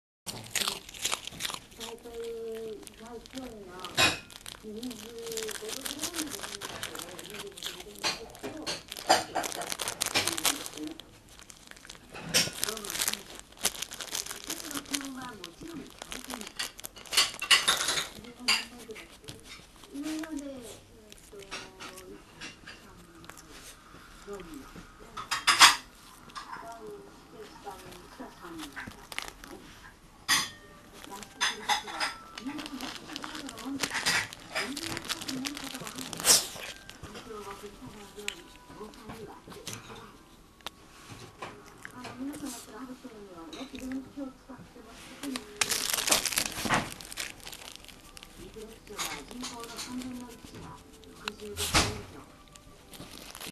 せんべい
せんべい美味しいねぇ☆たろうは袋をあけて舌で取り出して食べます